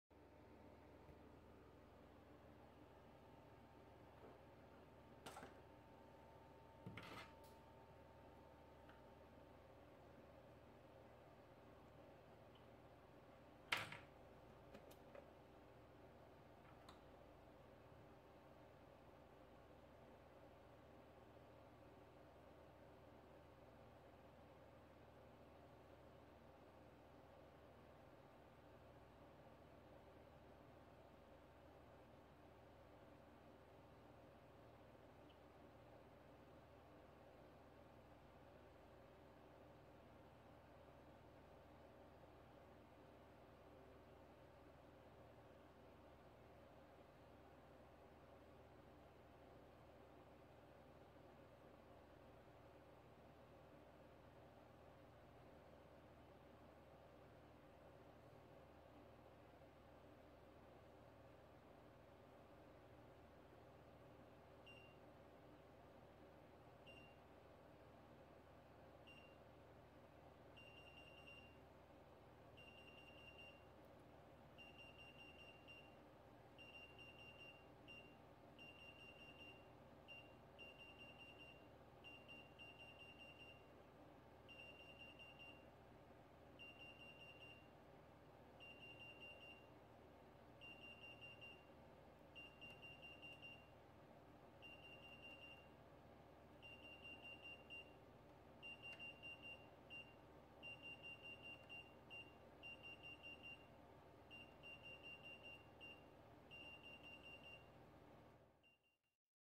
Workaraund for Travaglini SSC-T2-A
This panel has faulty touchscreen. And this is workaround for that temporarily.